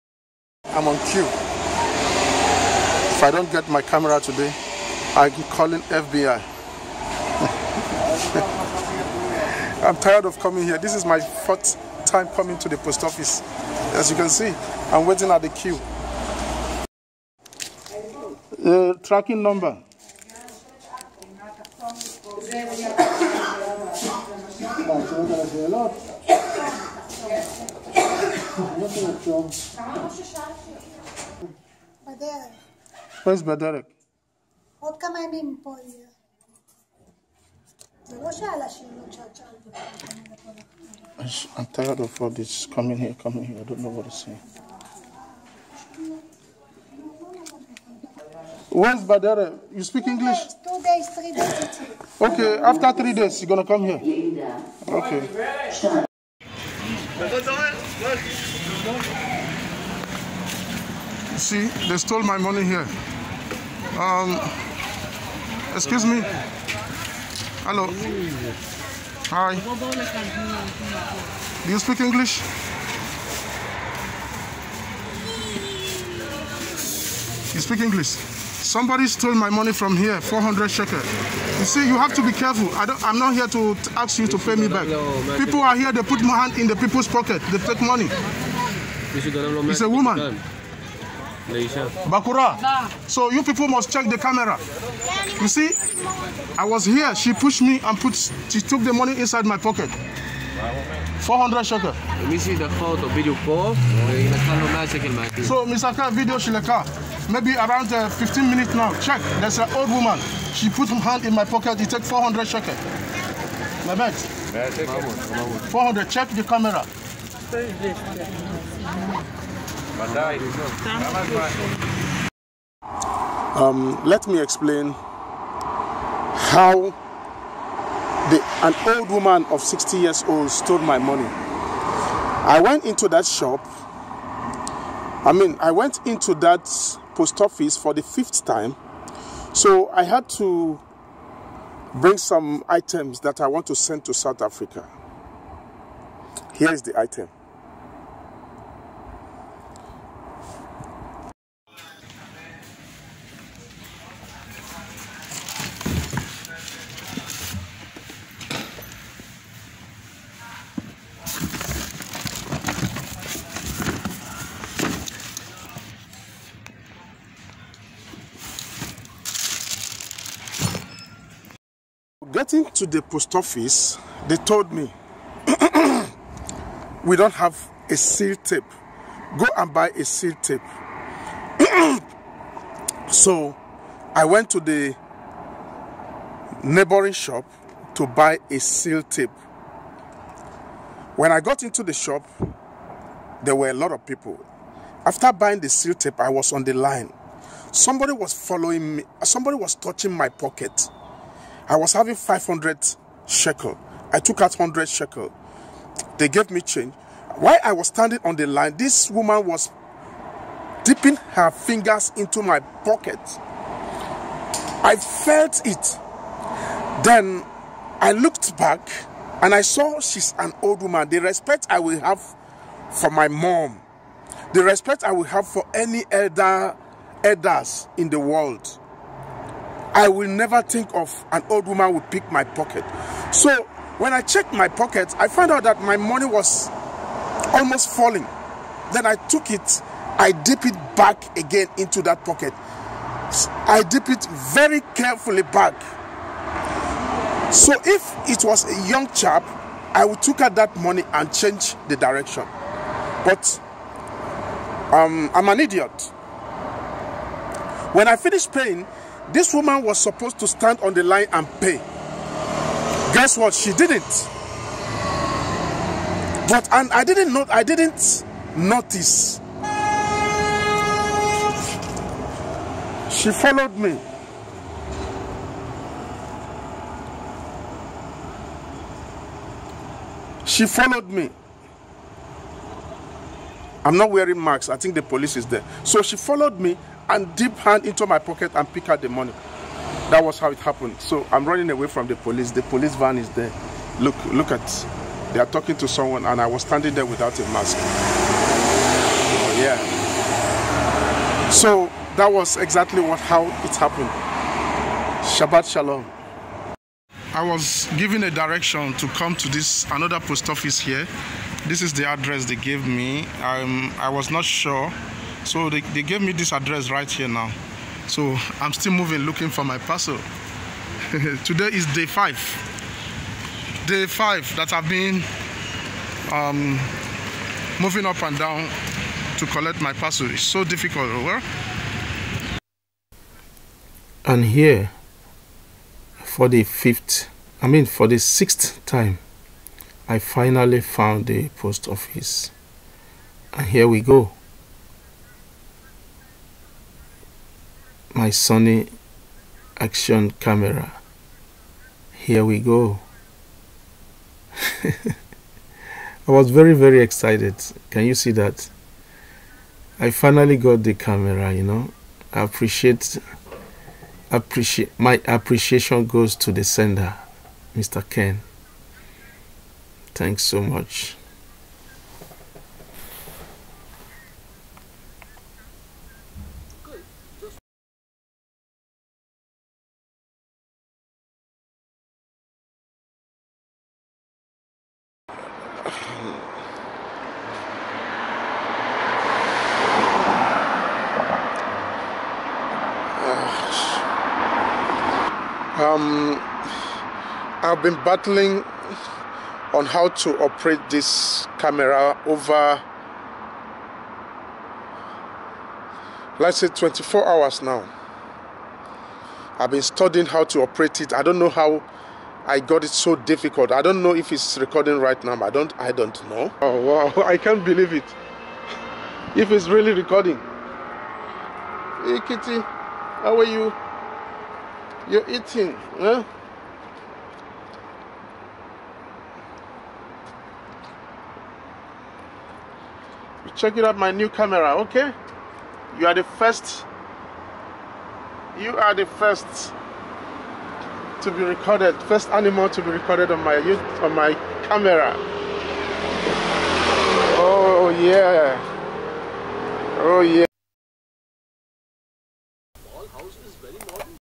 MY SONY ACTION CAMERA ARRIVED ,GRANDMA ROBBED ME CASH
MY SONY ACTION CAMERA ARRIVED WITH STRESS, AND I WAS ROBBED BY AN OLD GRANDMA